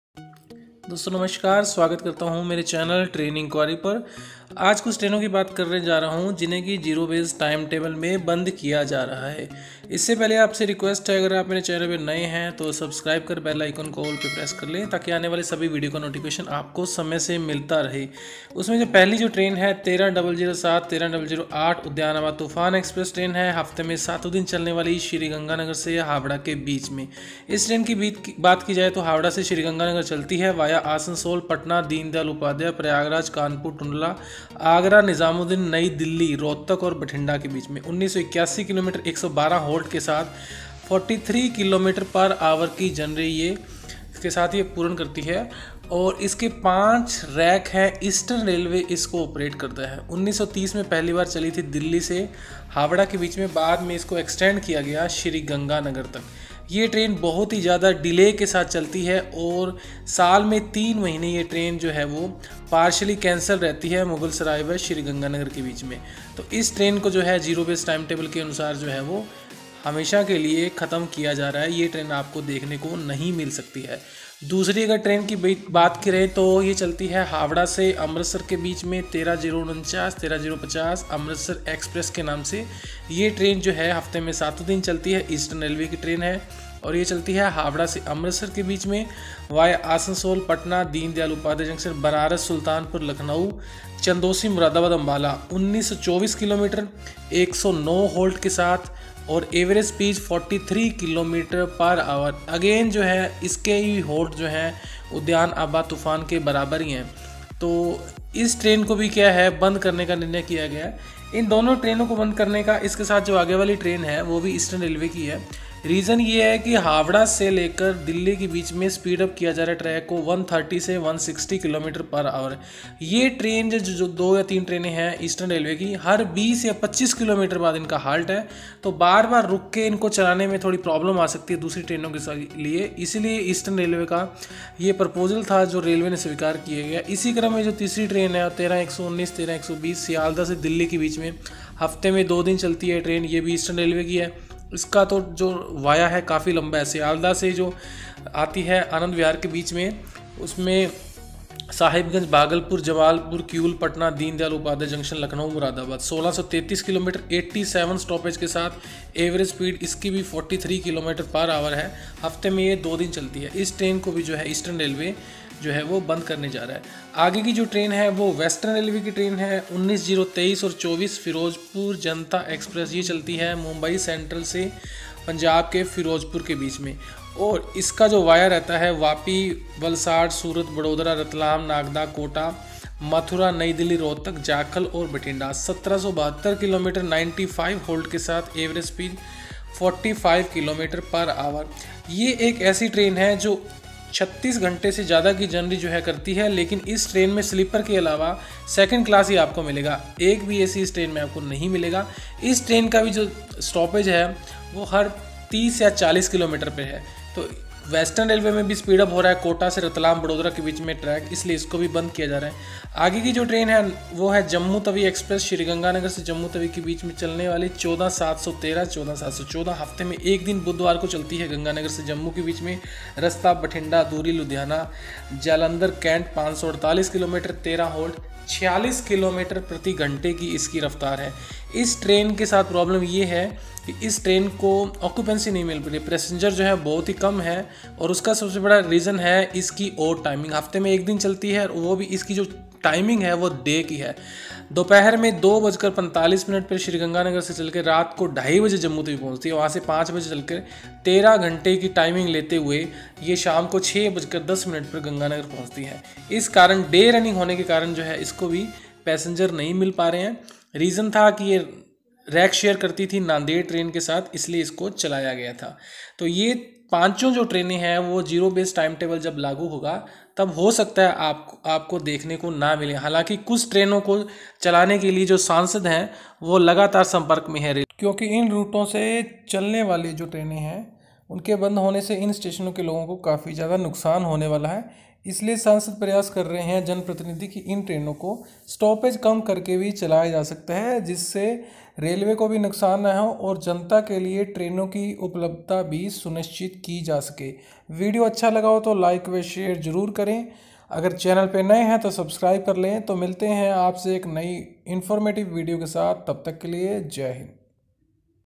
List of train Permanently cancelled run in Northern railway.
List of cancelled trains of eastern railway.
northern railway ki trains permanent cancelled.
Udyan abha toofan exp 13007.
Ferojpur janta exp.19023.
Amritsar howrah exp.13049.
13119 sealdah Anvt exp.
Sgnr Jat exp 14713.